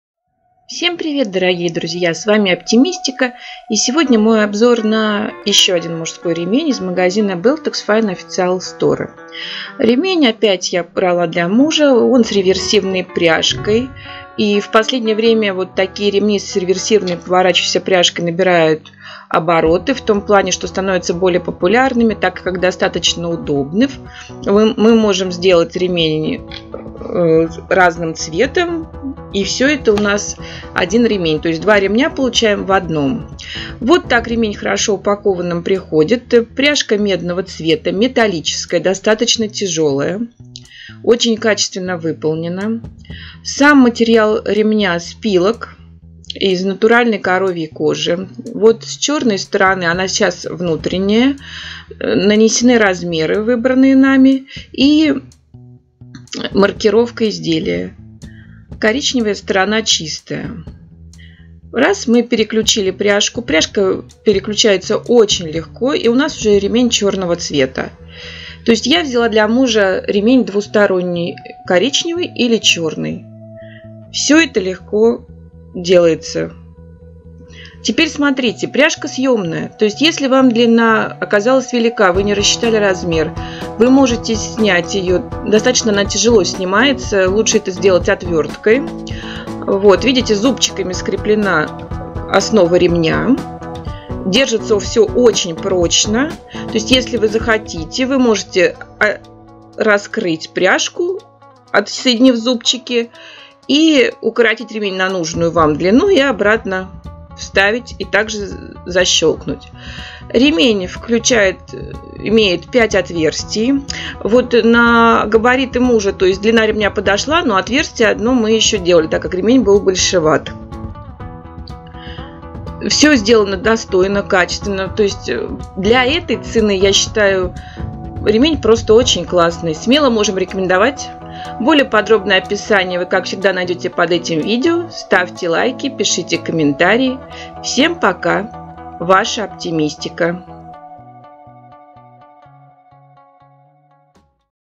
Мужской ремень с реверсивной пряжкой из beltox fine Official Store
Качественный мужской ремень
Из beltox fine Official Store

Отличная возможность получить два ремня в одном за счет реверсивной пряжки.

Ремни с реверсивной пряжкой давно уже набирают популярность, и я не оказалась исключением. Это уже не первый ремень с подобной пряжкой, приобретенный мною в этом магазине. Но он отличного качества, и мужу нравится, поэтому я заказала еще один.

Доставка месяц, тремномер отслеживался, посылку получала на почте.
Ремень приходит хорошо упакованным в почтовый пакет с пупыркой. Дополнительно сам ремень упакован в узкий полиэтиленовый чехол, а пряжка, во избежание появления царапин, была упакована в чехол из вздутого пенопласта. Поэтому товар пришел без повреждений.

Ремень полностью соответствует описанию на странице продавца. Я выбрала для мужа ремень в коричнево-черных цветах с медной пряжкой.

Пряжка качественная, поворотная, с ее помощью легко менять сторону ремня на лицевую. Пряжка съемная. Если вы вдруг не угадали с размером ремня, то можно разжать зубчики и, достав ремень, укоротить его на необходимую длину, после чего закрепить ремень обратно. Зубчики очень прочные, поэтому разжимать их лучше всего как миниум отверткой.

На черной (внутренней) стороне ремня указан размер ремня. На мужа мы выбрали размер 115(100). Длина ремня оказалась хорошей, а вот отверстий (их пять) не хватило, ремень сел слишком свободно, и муж специальным прибором сделал еще одно.

Качество исполнения на высоте. Отстрочки все ровные. Пять отверстий для корректировки размера. Материал ремня спилок.

Смело мужем рекомендовать.

Ссылка на товар

Всем добра)))
Ваша OptiMist_i_ka